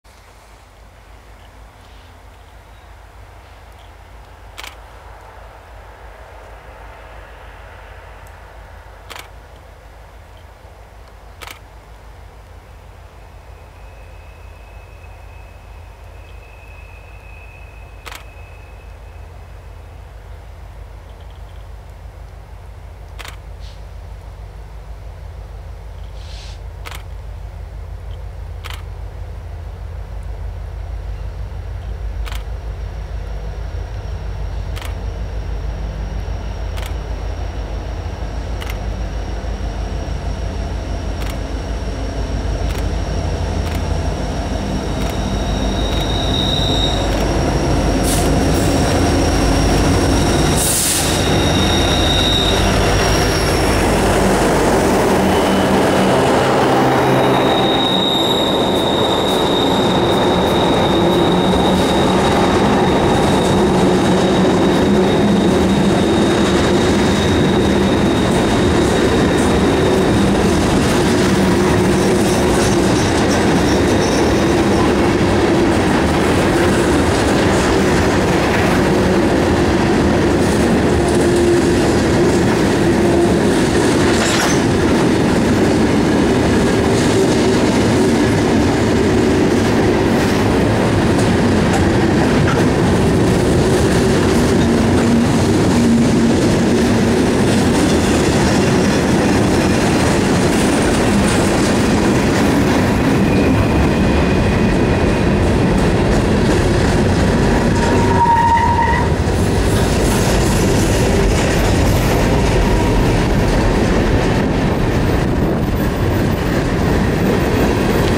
UP 1983 at Bloomer Cut
Shot Jan 2, 2011.  Bloomer Cut west of Auburn CA.